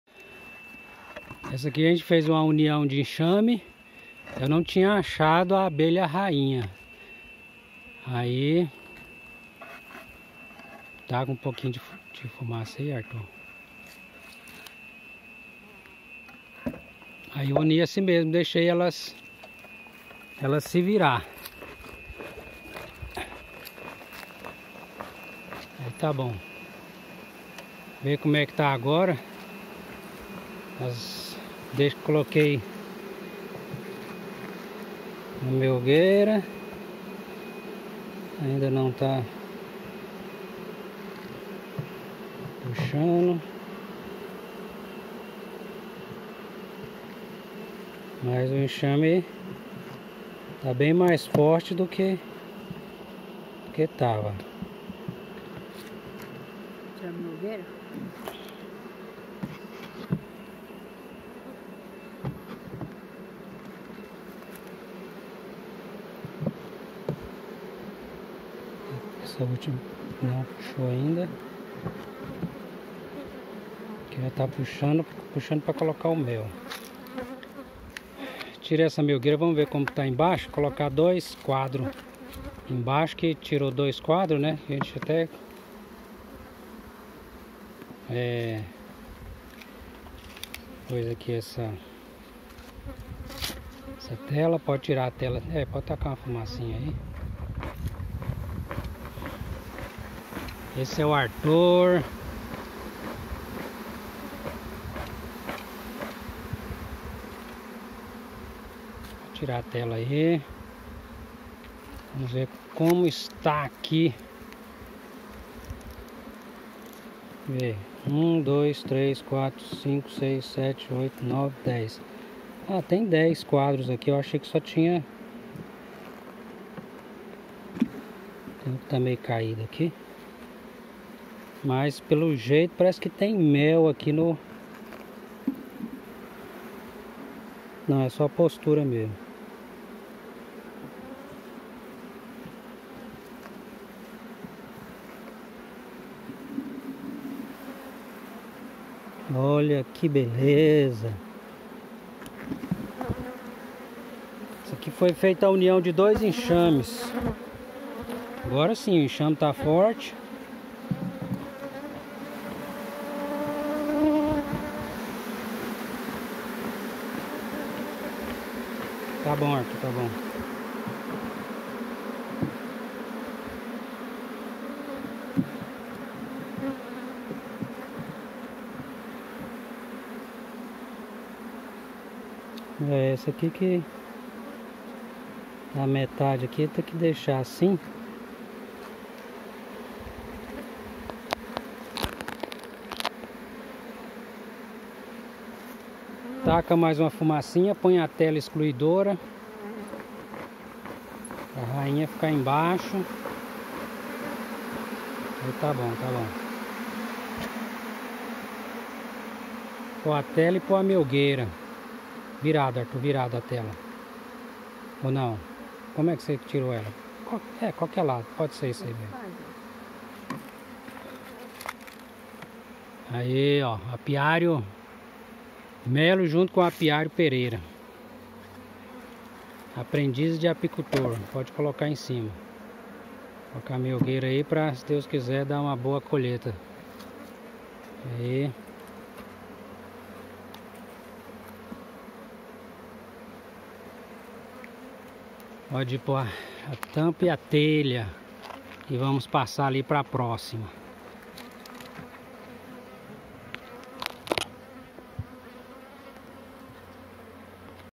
Revisão depois de uns dias da união de enxames - Apiário Melo
Abelhas PONTALINA GO